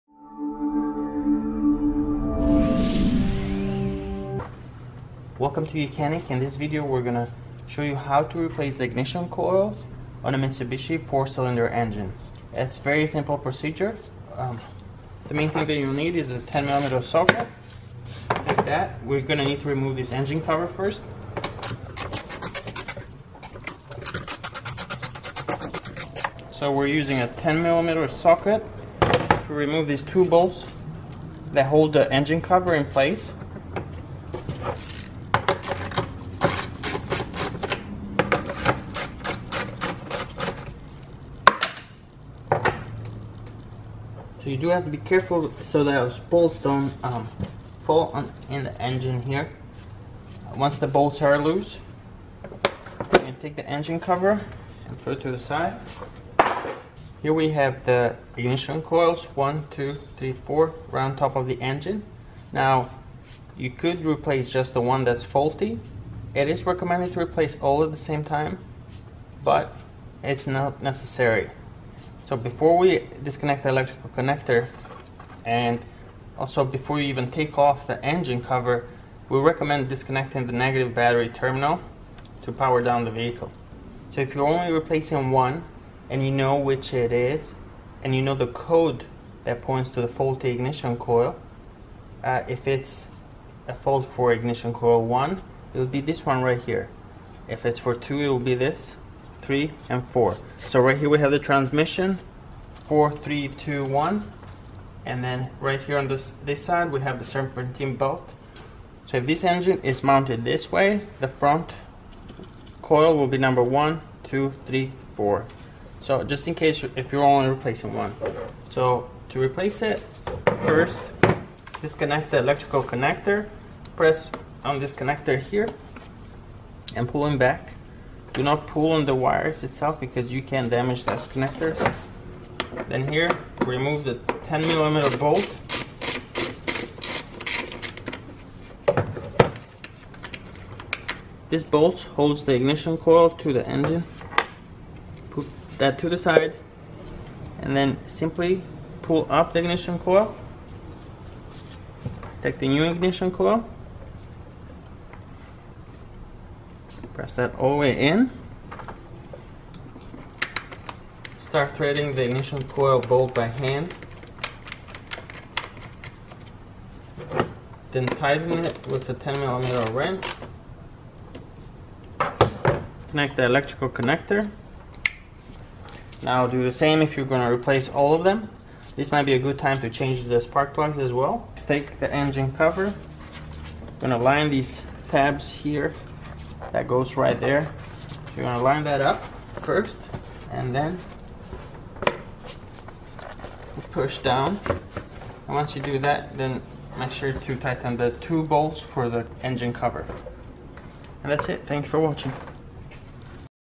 Mitsubishi 4-Cylinder Engine Ignition Coil Replacement Procedure
This provides a comprehensive tutorial on replacing the ignition coil on a Mitsubishi vehicle. The video explains the importance of a functioning ignition coil for optimal engine performance and fuel efficiency. It demonstrates the step-by-step process of safely removing the old coil, installing a new one, and properly testing and verifying the new coil using basic tools and techniques. The tutorial covers important factors such as identifying the location of the ignition coil assembly and retaining bolts, properly disconnecting and reconnecting the electrical connectors, and properly aligning and securing the new coil. Additionally, the video provides helpful tips on troubleshooting common issues that may arise during the replacement process, such as faulty wiring or damaged connectors. Overall, this video is informative, easy to follow, and a valuable resource for Mitsubishi owners looking to perform basic maintenance on their car's ignition coil system.

How to Replace the Ignition Coil on a Mitsubishi